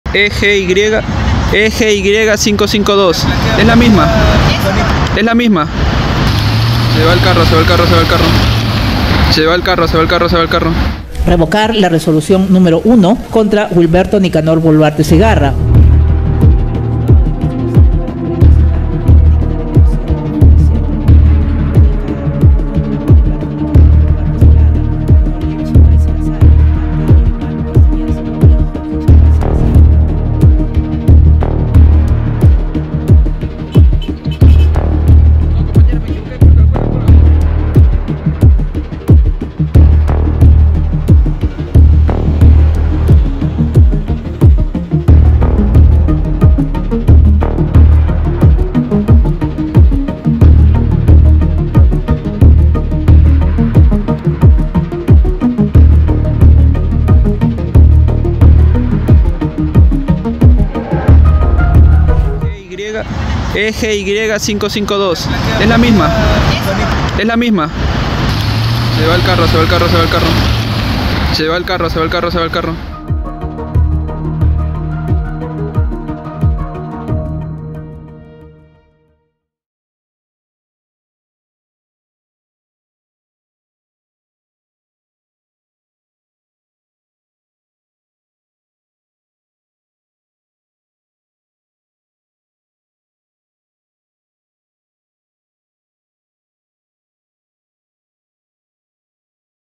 Captan VEHÍCULO de PRESIDENCIA en CASA de NICANOR BOLUARTE tras ser LIBERADO
Una vez que Nicanor Boluarte salió en libertad, un equipo periodístico de La República llegó hasta la vivienda del hermano de Dina Boluarte. Sin embargo, grande fue la sorpresa al encontrar un vehículo del Despacho Presidencial en los exteriores de su domicilio en San Borja. Tras ser grabado, la unidad emprendió la fuga, por lo que varias interrogantes surgieron, debido a que estas unidades solo puede estar en actividades del Gobierno.